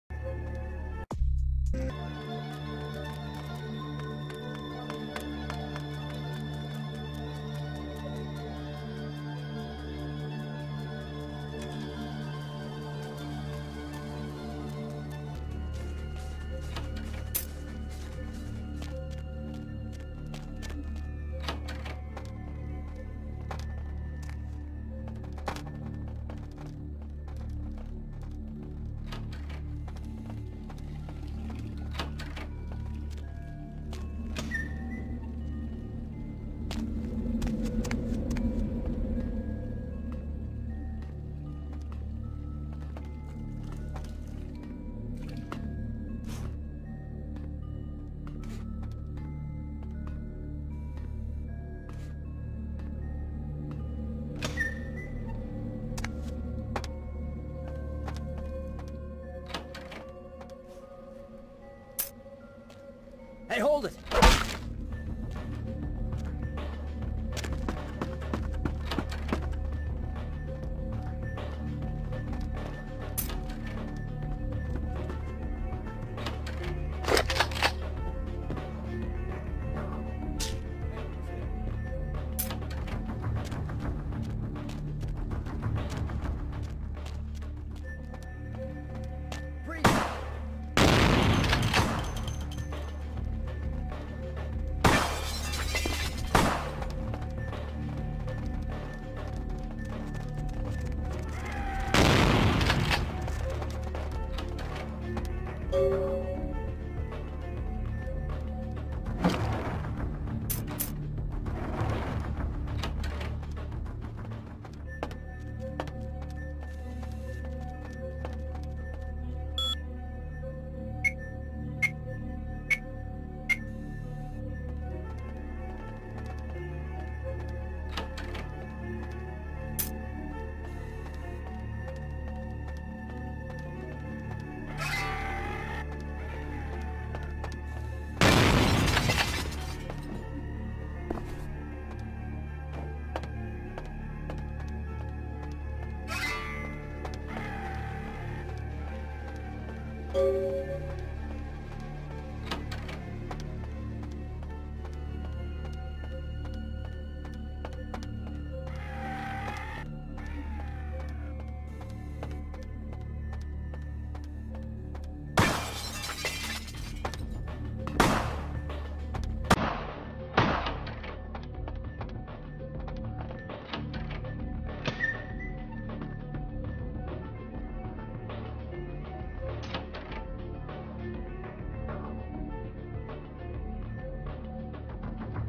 Museum No Skill No Kill
UP 10.4 Basic Patch, no kill bonus, no disciplines or blood used, very low skill character - 1 sneaking, 1 ranged, 2 lockpicking. NOTE: In the newer UPs the last security door has been locked again, as it was originally.